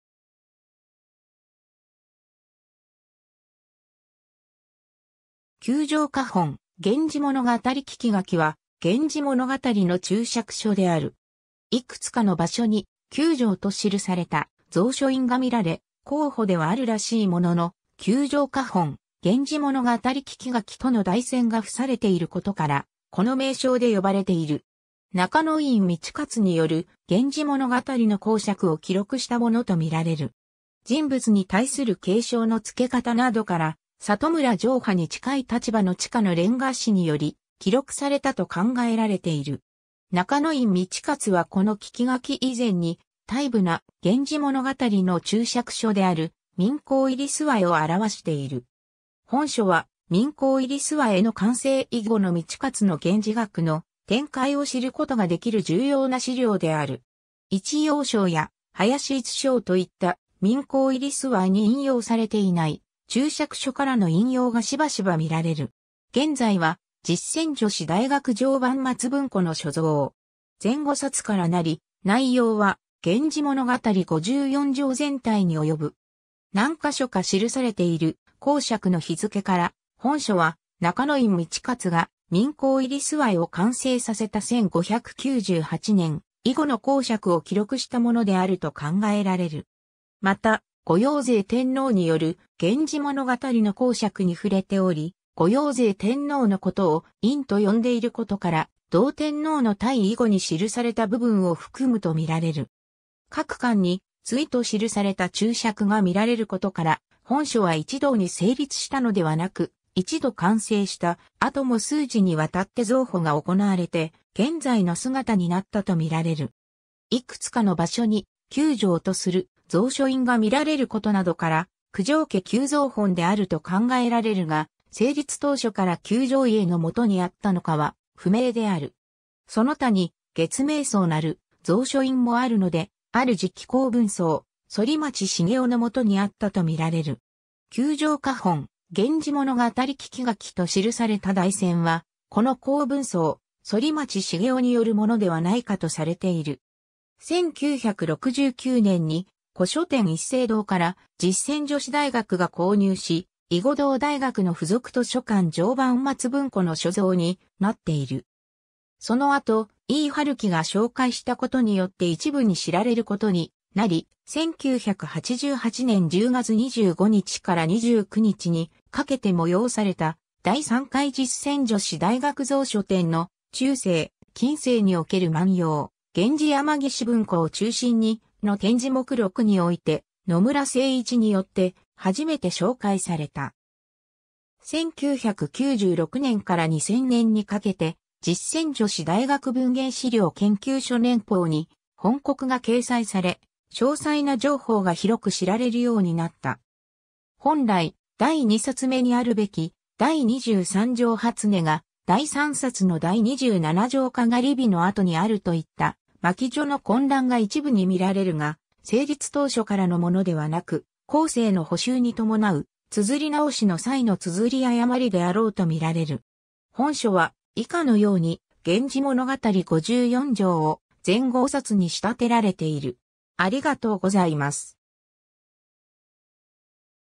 九条家本源氏物語聞書
『九条家本源氏物語聞書』（くじょうけほんげんじものがたりききがき）は、『源氏物語』の注釈書である。
 いくつかの場所に「九条」と記された蔵書印が見られ、後補ではあるらしいものの「九条家本源氏物語聞書」との題箋が付されていることからこの名称で呼ばれている。
 中院通勝による『源氏物語』の講釈を記録したものと見られる。
人物に対する敬称の付け方などから、里村紹巴に近い立場の地下の連歌師により記録されたと考えられている。
 中院通勝はこの聞書以前に、大部な『源氏物語』の注釈書である『岷江入楚』を著している。
本書は『岷江入楚』の完成以後の通勝の源氏学の展開を知ることが出来る重要な資料である。
『一葉抄』や『林逸抄』といった、『岷江入楚』に引用されていない注釈書からの引用がしばしば見られる。
 現在は実践女子大学常磐松文庫の所蔵。
全5冊からなり、内容は『源氏物語』54帖全体に及ぶ。
 何カ所か記されている講釈の日付から、本書は中院通勝が『岷江入楚』を完成させた1598年（慶長3年）以後の講釈を記録したものであると考えられる。
また、後陽成天皇による『源氏物語』の講釈に触れており、後陽成天皇のことを「院」と呼んでいることから、同天皇の退位（慶長16年3月27日（1611年5月9日））以後に記された部分を含むと見られる。
 各巻に「追」と記された注釈が見られることから、本書は一度に成立したのではなく、一度完成した後も数次にわたって増補が行われて現在の姿になったと見られる。
 いくつかの場所に「九条」とする蔵書印が見られることなどから、九条家旧蔵本であると考えられるが、成立当初から九条家のもとにあったのかは不明である。
その他に「月明荘」なる蔵書印もあるので、ある時期弘文荘反町茂雄のもとにあったと見られる。
「九条家本源氏物語聞書」と記された題箋は、この弘文荘反町茂雄によるものではないかとされている。
 1969年（昭和44年）に古書店一誠堂から実践女子大学が購入し、以後同大学の付属図書館常磐松文庫の所蔵になっている。
 その後伊井春樹が紹介したことによって一部に知られることになり、1988年（昭和63年）10月25日から29日にかけて催された『第3回実践女子大学蔵書展』の「中世・近世における万葉・源氏　山岸文庫を中心に」の展示目録において野村精一によって初めて紹介された。
1996年（平成8年）から2000年（平成12年）にかけて『実践女子大学文芸資料研究所 年報』に翻刻が掲載され、詳細な情報が広く知られるようになった。
 本来第2冊目にあるべき第23帖 初音（第22帖 玉鬘の次巻）が第3冊の第27帖 篝火の後にあるといった巻序の混乱が一部に見られるが、成立当初からのものではなく、後世の補修に伴う綴り直しの際の綴り誤りであろうと見られる。
 本書は以下のように『源氏物語』54帖を全5冊に仕立てられている。